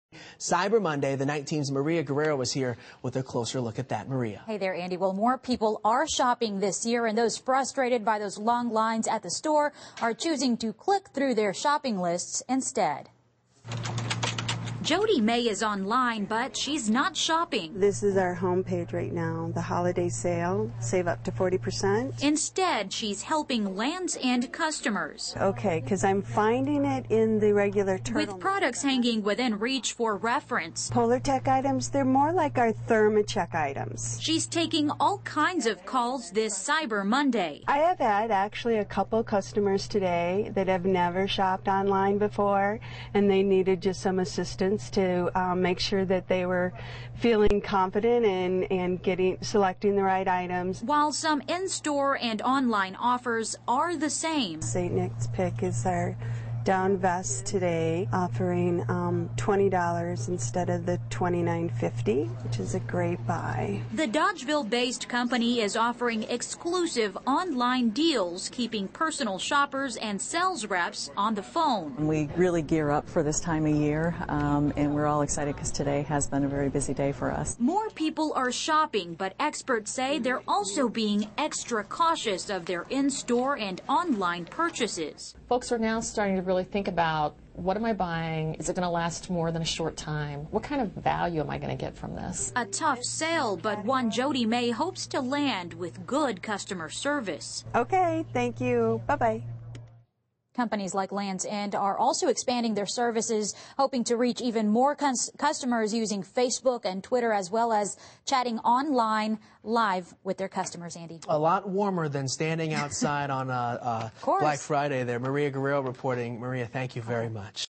Businesses Seek To Lure In Customers On Cyber Monday
Its been a busy weekend for holiday shoppers, and new numbers show some
growth in sales over last year, according to industry watchers. Maria
Guerrero reports